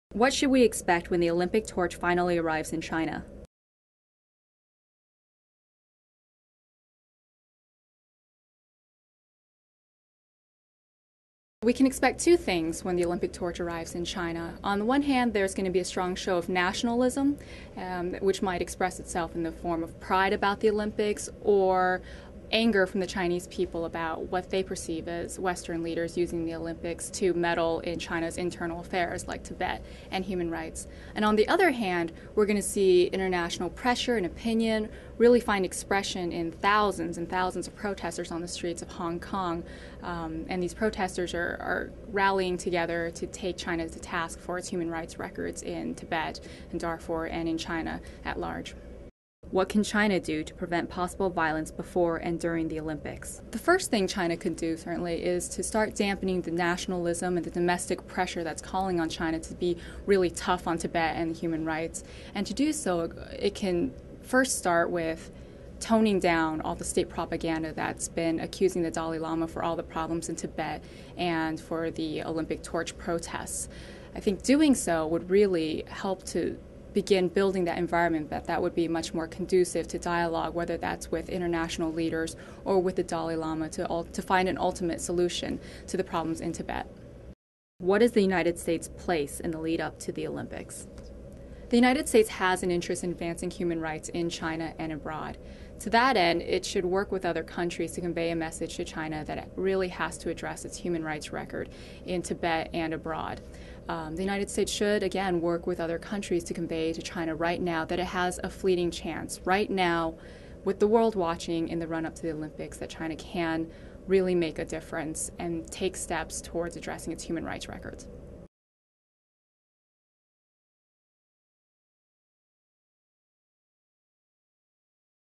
The U.S. & The 2008 Beijing Olympics -ASK THE EXPERT- ...
What should we expect when the Olympic torch finally arrives in China? What can China do to prevent possible violence before and during the Olympics? What is the United States' place in the lead-up to the Olympics?